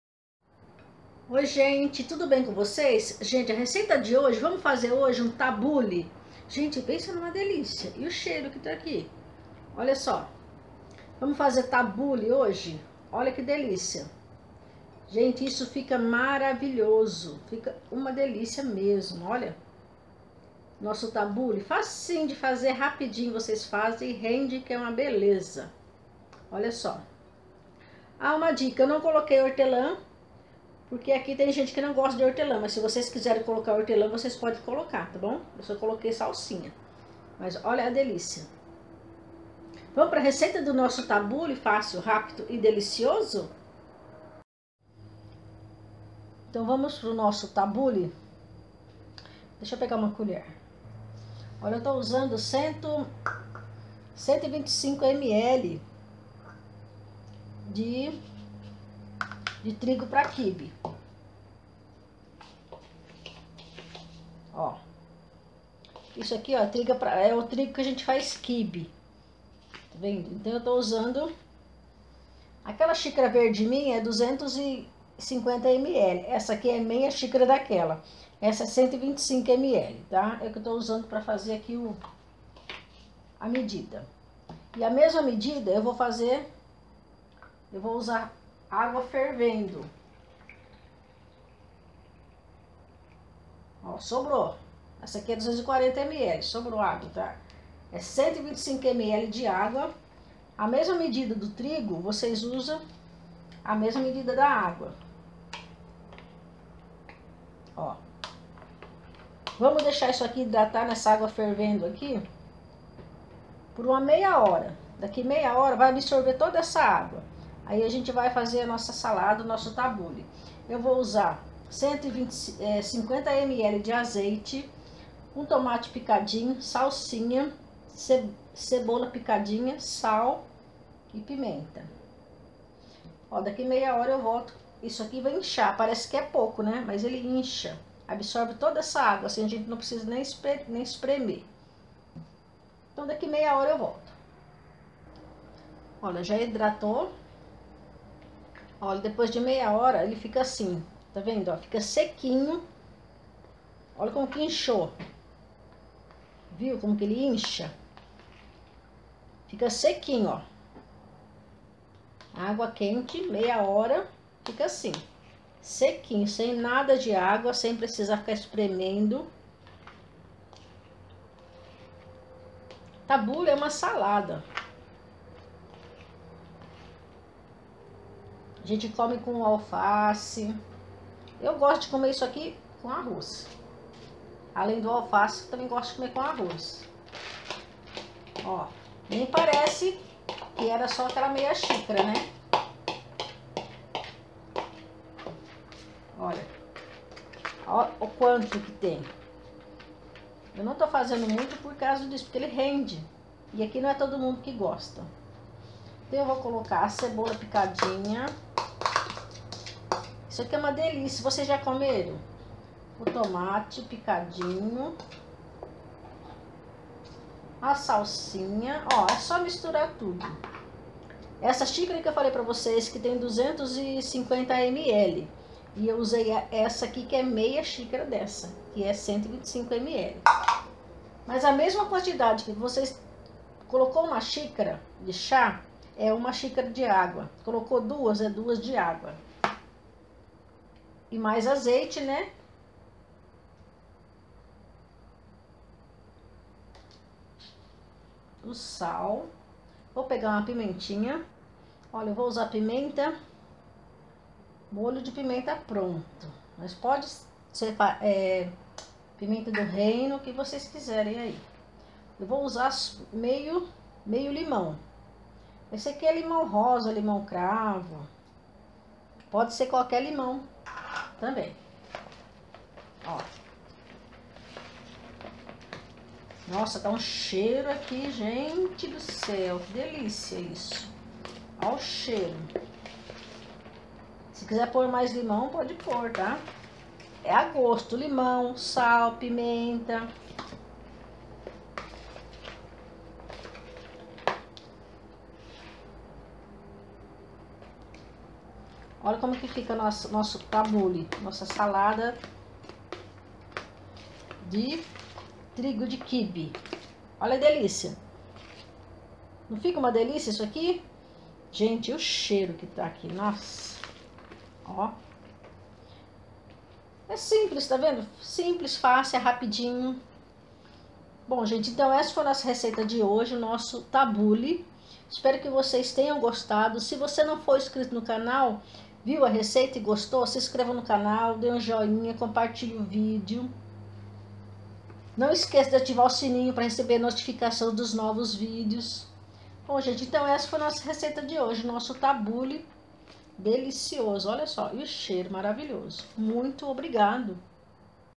Como fazer tabule fácil rápido e delicioso
ingredientes
Meia xícara de 125 ml de trigo para kibe
125 ml de água quente
1 tomate picadinho
1 cebola picadinha
50 ml de azeite
sal a agosto
Pimenta a gosto
hortelã a agosto
 salsinha a gosto
 limão a gosto